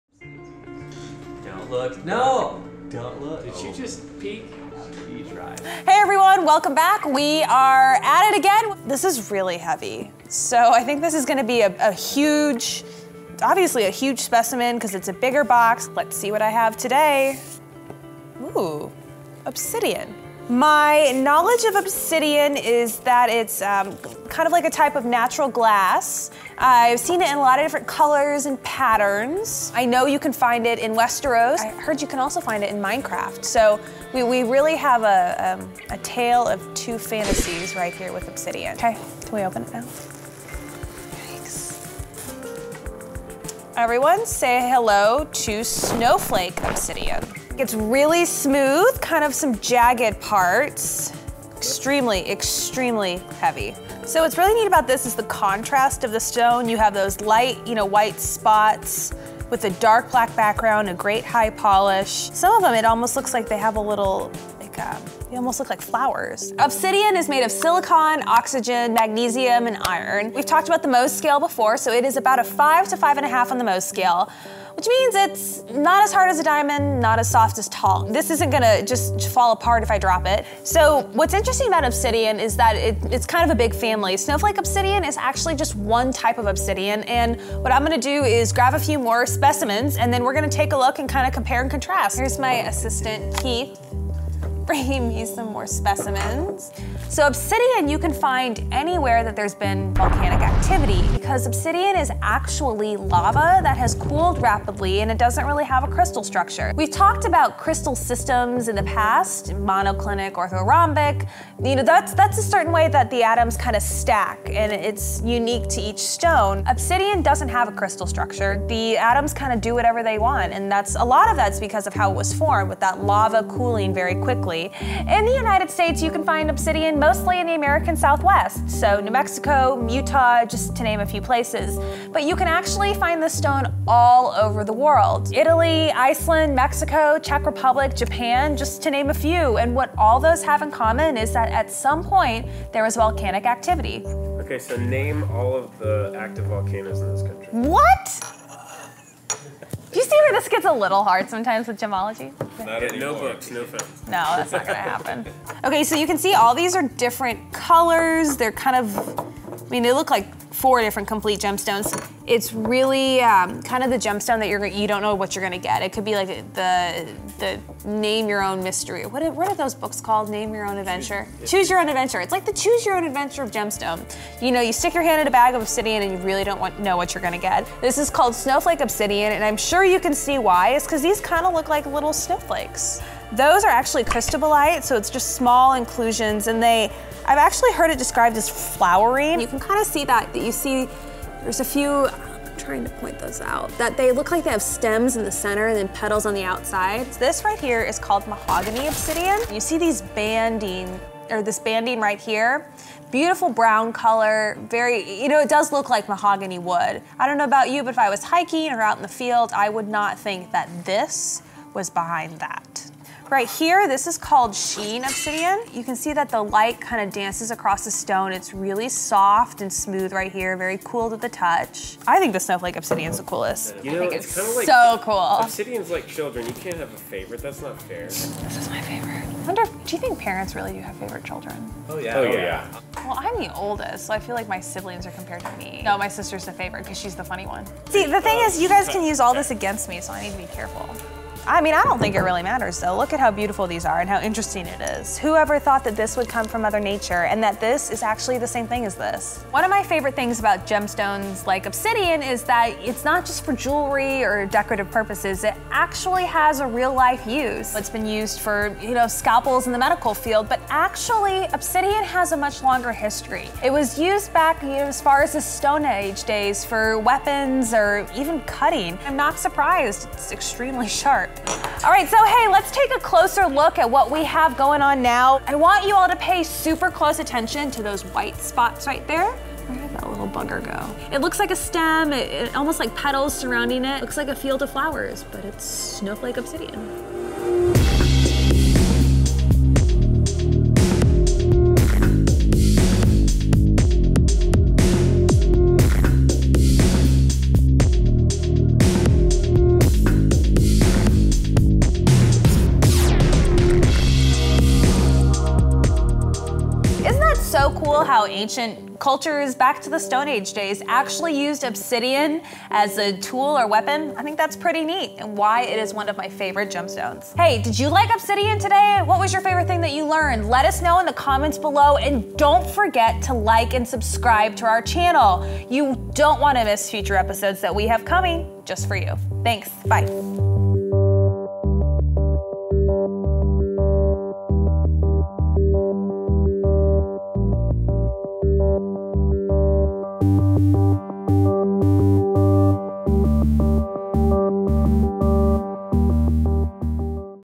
Obsidian: Lava... to Gemstone?
Obsidian can be found wherever there has been volcanic activity. That's right... obsidian is just lava that has cooled very, very quickly! Natalie unboxes a couple different types and explains everything you need to know about obsidian.